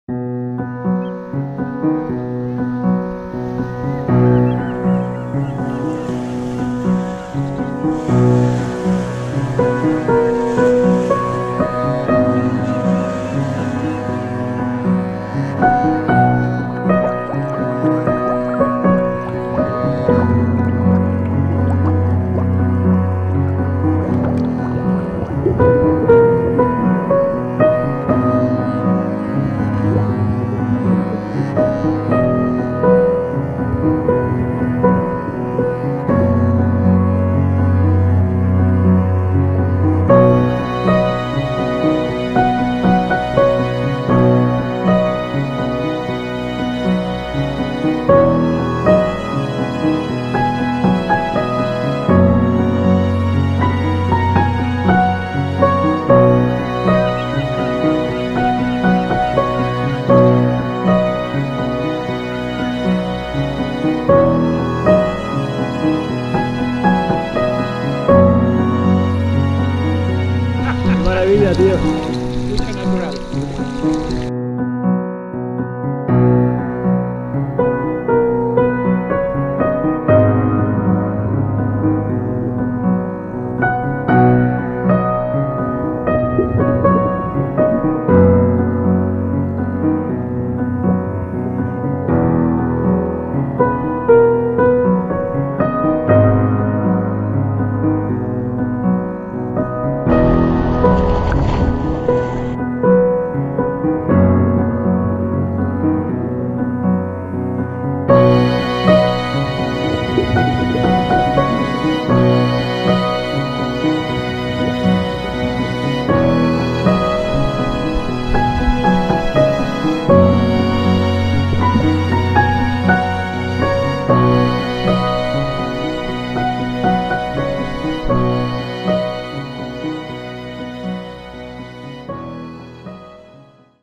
Swim Los Canos to Barbate
Como parte del reto de nadar de Conil a Tarifa en 16 etapas aquí hacemos la etapa 6, unos 6,000m. entre Los Caños y Tarifa pasando por el magnifico acantildao del Parque Natural de La Breña.